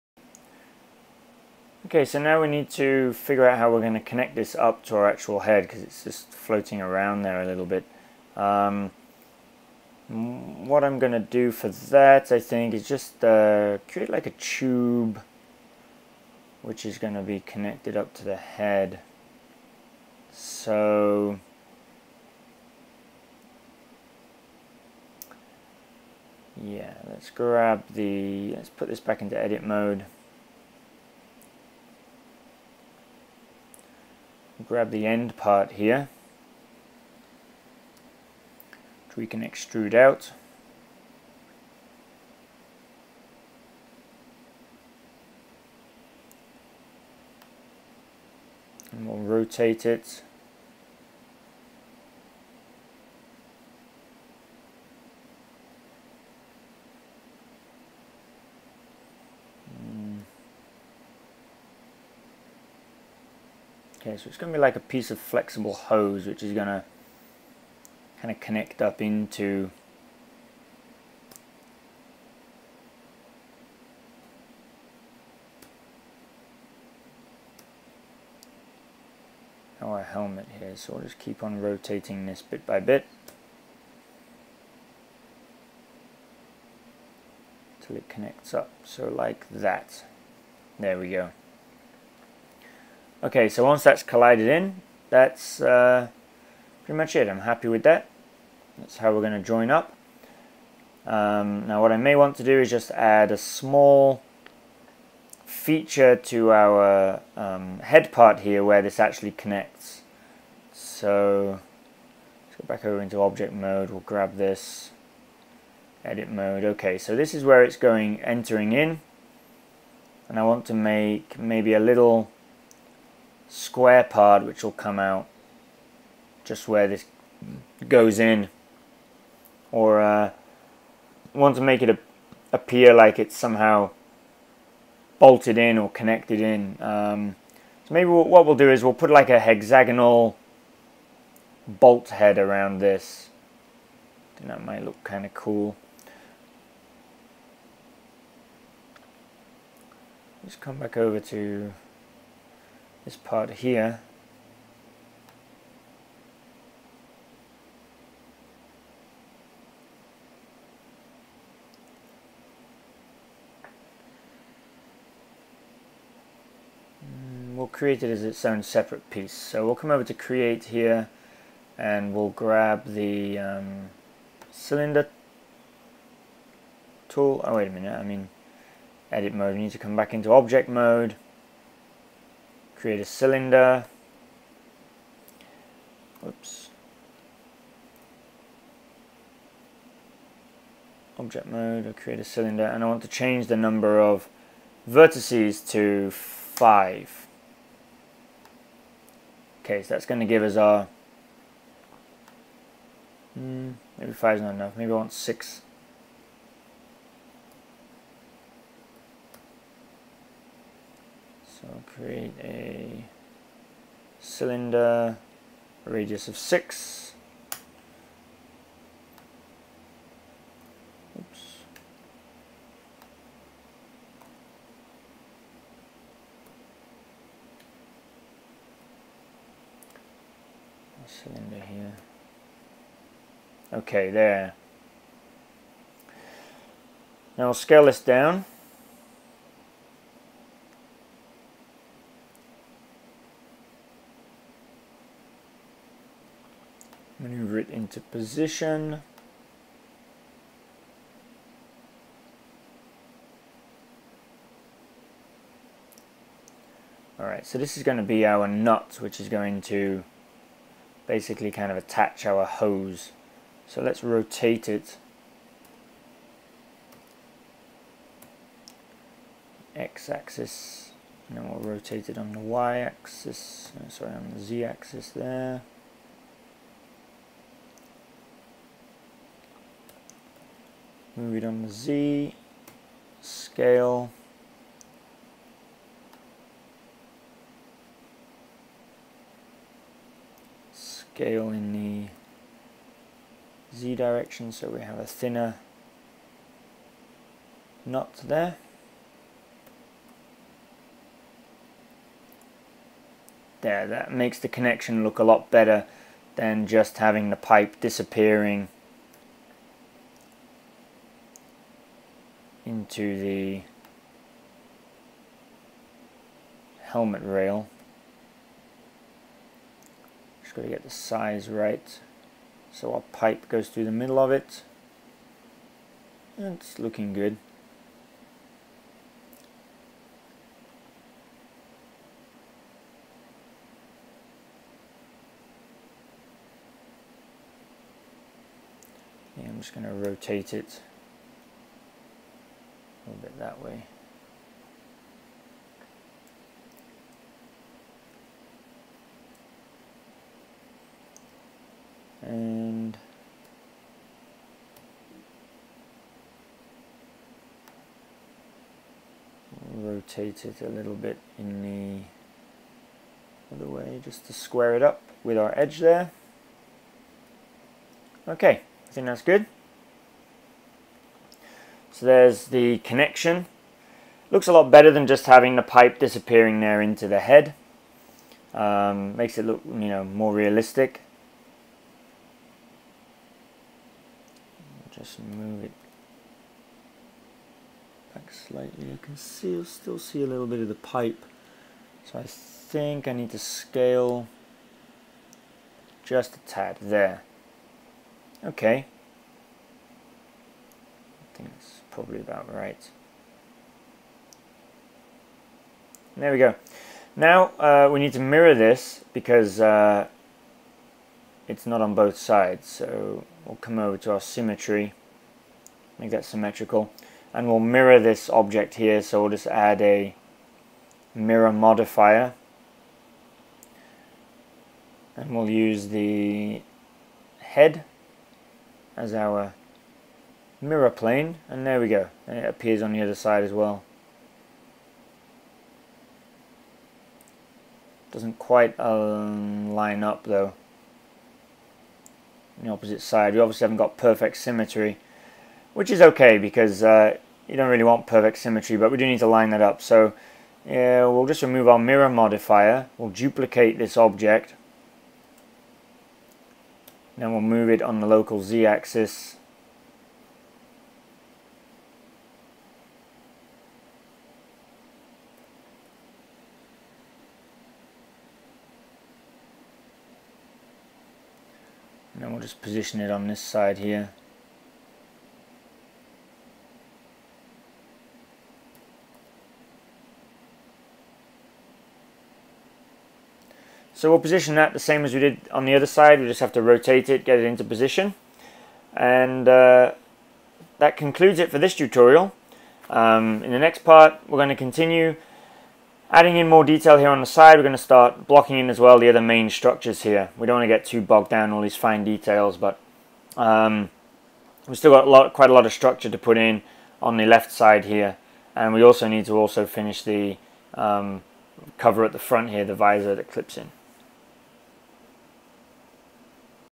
Sci Fi Helmet - Modelling Tutorial 5
In this tutorial we will do some finer detail modelling on our hydraulic hose, convert the nurbs surface to an editable mesh and manipulate it using Extrusion tools.